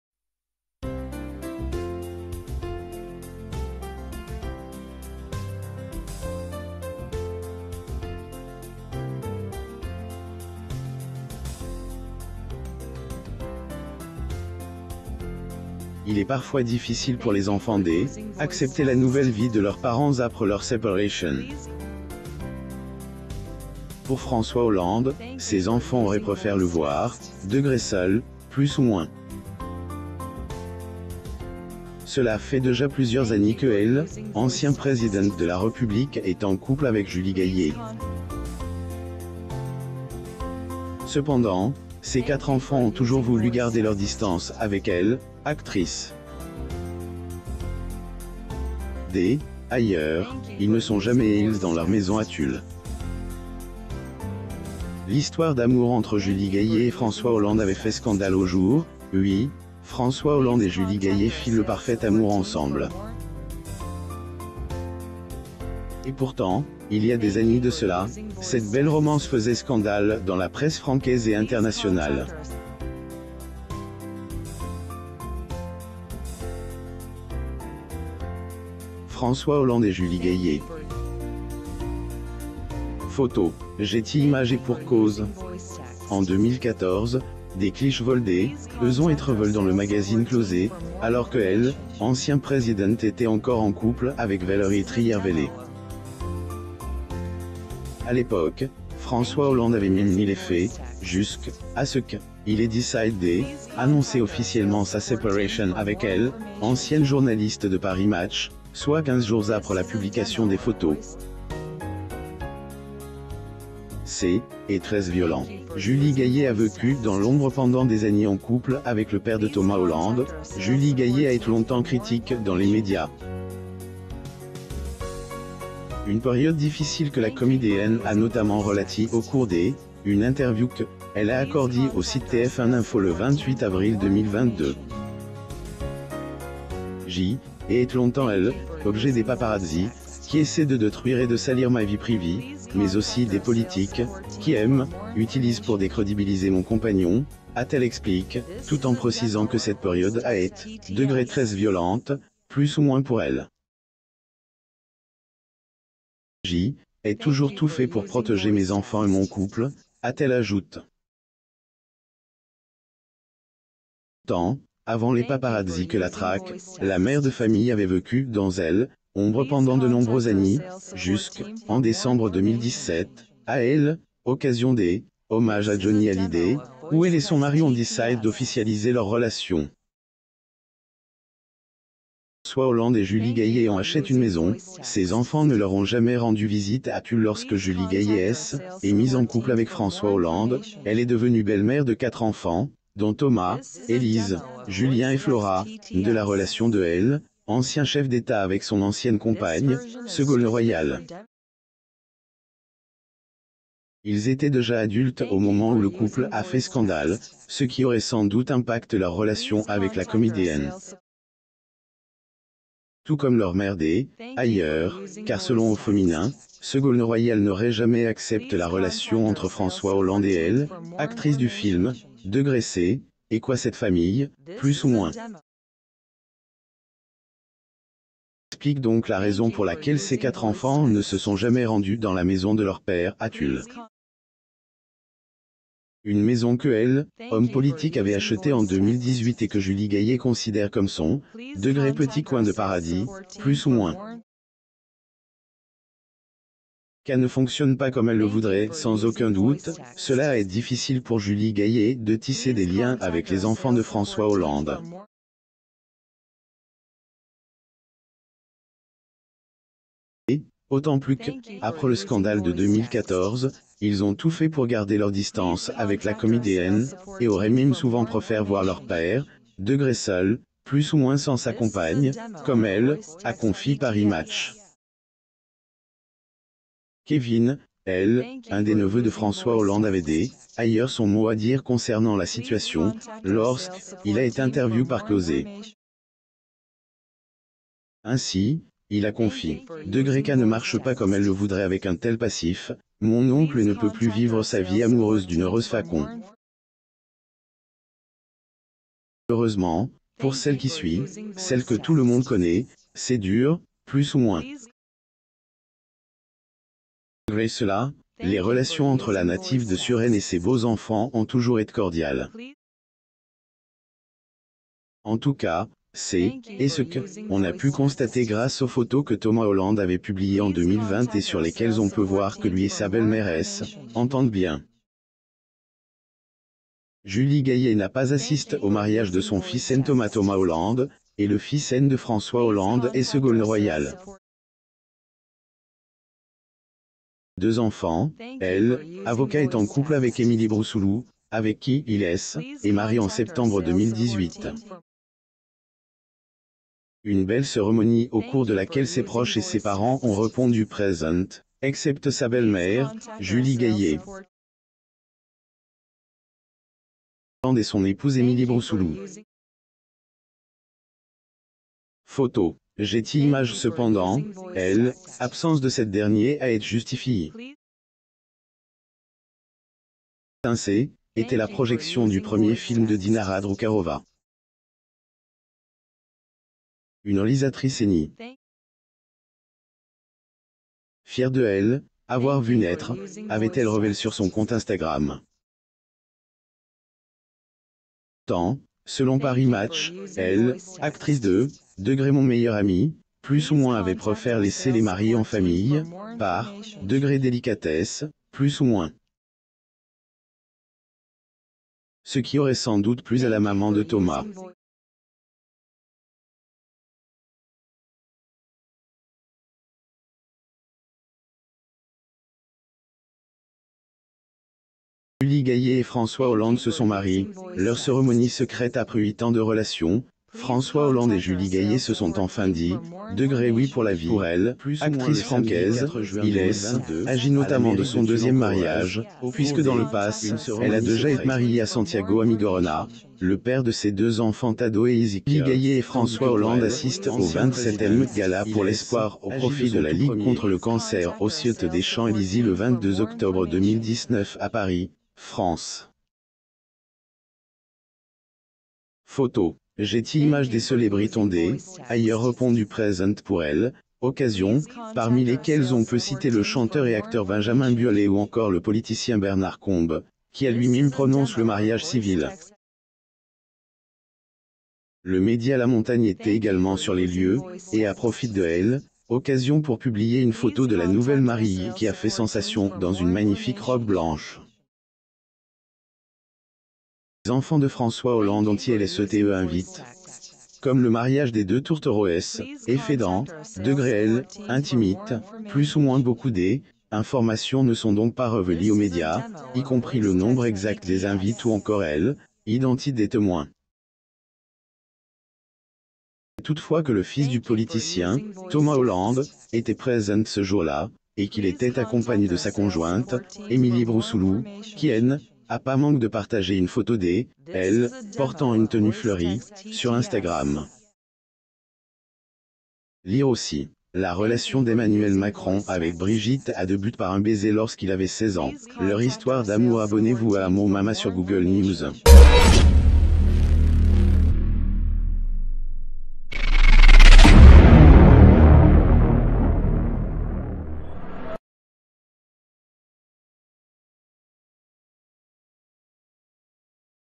Les enfants de François Hollande ont préféré le voir "seul" sans Julie Gayet - Ils ne sont jamais
Les enfants de François Hollande ont préféré le voir "seul" sans Julie Gayet - Ils ne sont jamais |Invicta Men's Pro Diver Stainless Steel Quartz Watch . Please help me with an order to maintain the system, thanks a lot.| Il est parfois difficile pour les enfants d’accepter la nouvelle vie de leurs parents après leur séparation. Pour François Hollande, ces enfants auraient préféré le voir “seul”. Cela fait déjà plusieurs années que l’ancien président de la République est en couple avec Julie Gayet. Cependant, ses quatre enfants ont toujours voulu garder leurs distances avec l’actrice. D’ailleurs, ils ne sont jamais allés dans leur maison à Tulle. L'HISTOIRE D'AMOUR ENTRE JULIE GAYET ET FRANÇOIS HOLLANDE AVAIT FAIT SCANDALE Aujourd’hui, François Hollande et Julie Gayet filent le parfait amour ensemble. Et pourtant, il y a des années de cela, cette belle romance faisait scandale dans la presse française et internationale. François Hollande et Julie Gayet. | Photo : Getty Images Et pour cause ? En 2014, des clichés volés d’eux ont été révélés dans le magazine Closer, alors que l’ancien président était encore en couple avec Valérie Trierweiler. À l'époque, François Hollande avait même nié les faits, jusqu’à ce qu’il ait décidé d’annoncer officiellement sa séparation avec l’ancienne journaliste de Paris Match, soit 15 jours après la publication des photos. "C’EST TRÈS VIOLENT" : JULIE GAYET A VÉCU DANS L'OMBRE PENDANT DES ANNÉES En couple avec le père de Thomas Hollande, Julie Gayet a été longtemps critiquée dans les médias. Une période difficile que la comédienne a notamment relatée au cours d’une interview qu’elle a accordée au site TF1 info le 28 avril 2022. "J’ai été longtemps l’objet des paparazzis, qui essaient de détruire et de salir ma vie privée, mais aussi des politiques, qui m’utilisent pour décrédibiliser mon compagnon", a-t-elle expliqué, tout en précisant que cette période a été “très violente” pour elle. “J’ai toujours tout fait pour protéger mes enfants et mon couple", a-t-elle ajouté. Pourtant, avant les paparazzis que la traquent, la mère de famille avait vécu dans l’ombre pendant de nombreuses années, jusqu’en décembre 2017, à l’occasion d’hommage à Johnny Hallyday, où elle et son mari ont décidé d'officialiser leur relation. QUAND FRANÇOIS HOLLANDE ET JULIE GAYET ONT ACHETÉ UNE MAISON - SES ENFANTS NE LEUR ONT JAMAIS RENDU VISITE À TULLE Lorsque Julie Gayet s’est mise en couple avec François Hollande, elle est devenue belle-mère de quatre enfants, dont Thomas, Élise, Julien et Flora, nés de la relation de l’ancien chef d'État avec son ancienne compagne, Ségolène Royal. Ils étaient déjà adultes au moment où le couple a fait scandale, ce qui aurait sans doute impacté leurs relations avec la comédienne. Tout comme leur mère d’ailleurs, car selon Auféminin, Ségolène Royal n'aurait jamais accepté la relation entre François Hollande et l’actrice du film “C’est quoi cette famille”. Cela explique donc la raison pour laquelle ses quatre enfants ne se sont jamais rendus dans la maison de leur père à Tulle. Une maison que l’homme politique avait achetée en 2018 et que Julie Gayet considère comme son “petit coin de paradis”. "ÇA NE FONCTIONNE PAS COMME ELLE LE VOUDRAIT" Sans aucun doute, cela a été difficile pour Julie Gayet de tisser des liens avec les enfants de François Hollande. D’autant plus qu’après le scandale de 2014, ils ont tout fait pour garder leurs distances avec la comédienne, et aurait même souvent préféré voir leur père “seul” sans sa compagne, comme l’a confié Paris Match. Kevin, l’un des neveux de François Hollande avait d’ailleurs son mot à dire concernant la situation, lorsqu’il a été interviewé par Closer. Ainsi, il a confié : “Ça ne marche pas comme elle le voudrait (...) Avec un tel passif, mon oncle ne peut plus vivre sa vie amoureuse d'une heureuse façon. Et malheureusement, pour celle qui suit, celle que tout le monde connaît, c'est dur”. Malgré cela, les relations entr